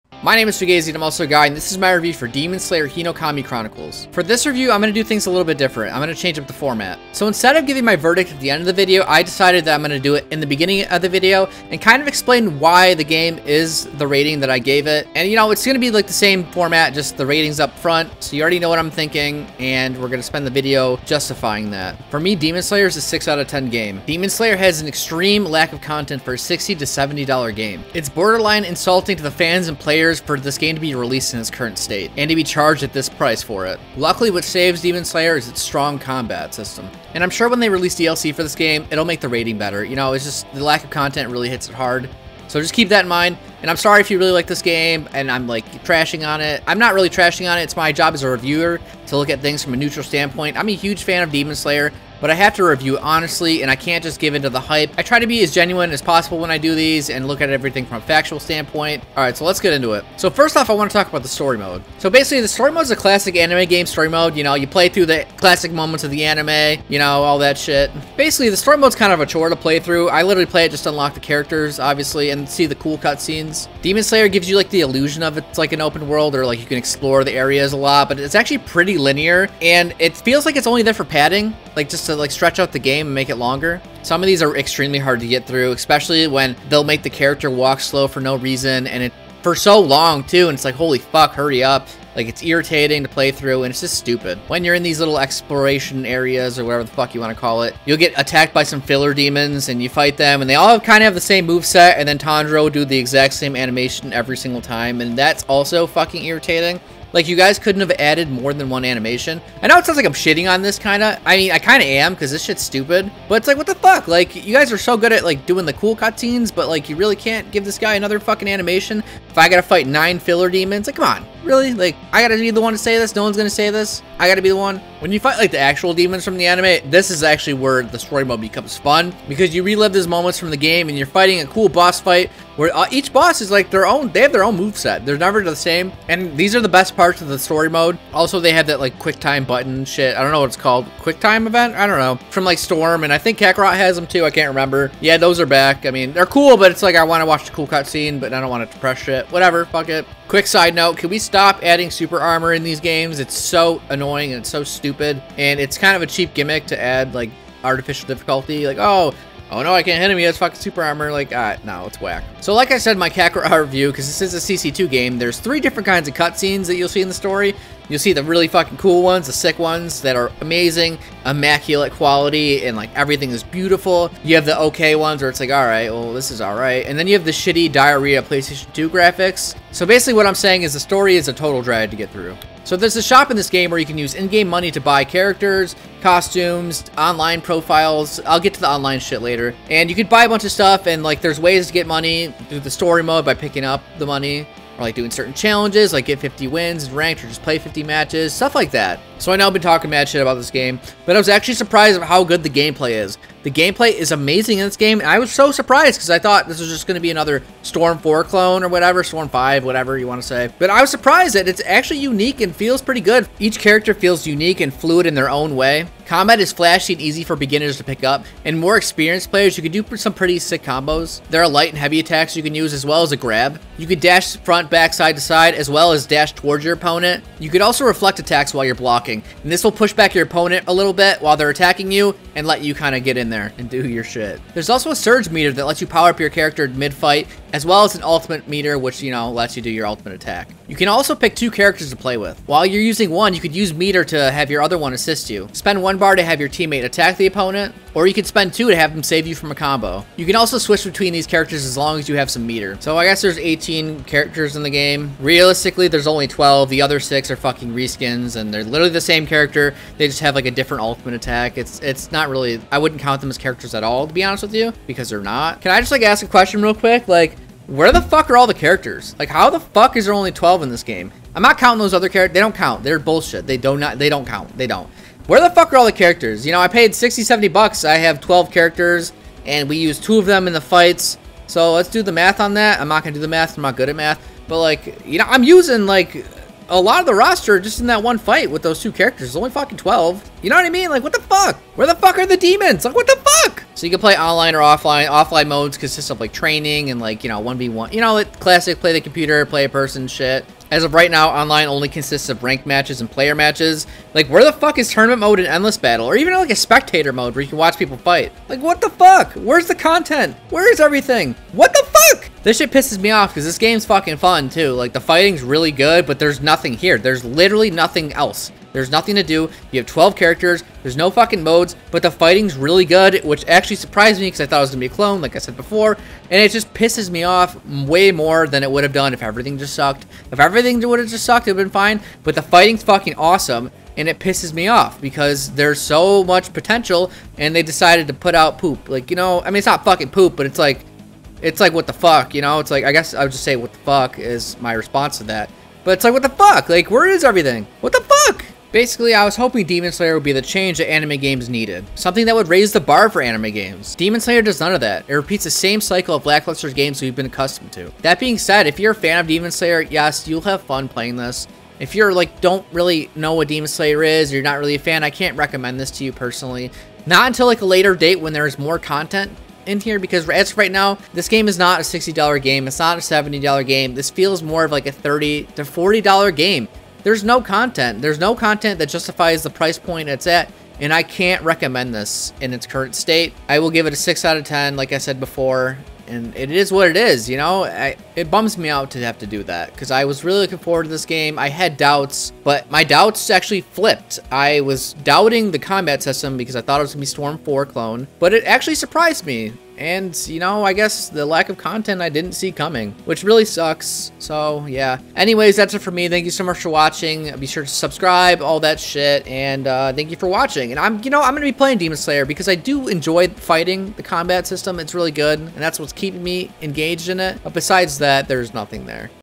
Demon Slayer The Hinokami Chronicles Review PC - Major Issues! (Where is All The Content???) Rant!
The extreme lack of content holds this game back so much. 18 fighters 6 of them are reskins so there's actually only 12. no demons. missing a ton of modes. i guess free dlc to come but in this state the is game bare bones.